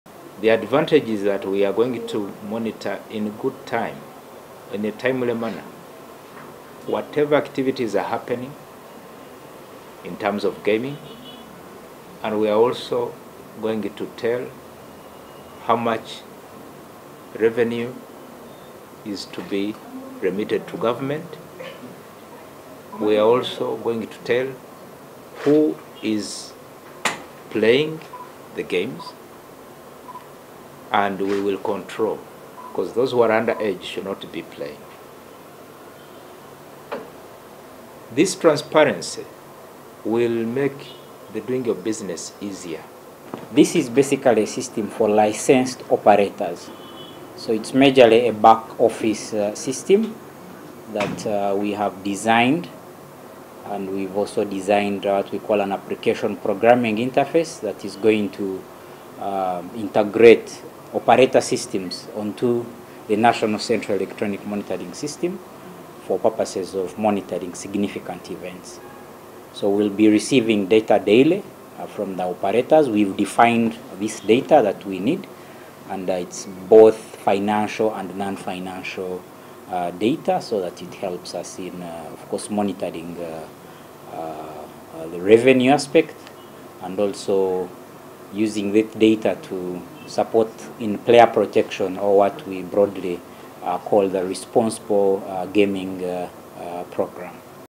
Regulatory board develops new measures
In a move to revolutionize gaming oversight, the Ministry of Finance and National Lotteries and Gaming Regulatory Board have started a transformative partnership with Comrade D.O.O, a technology company to improve tax compliance in the gaming industry. The collaboration has resulted in the development of a National Central Electronic Monitoring System that will address challenges in the gaming and betting industry, by offering solutions that range from enhanced compliance and increased revenue collection to improved enforcement and prioritizing responsible gaming.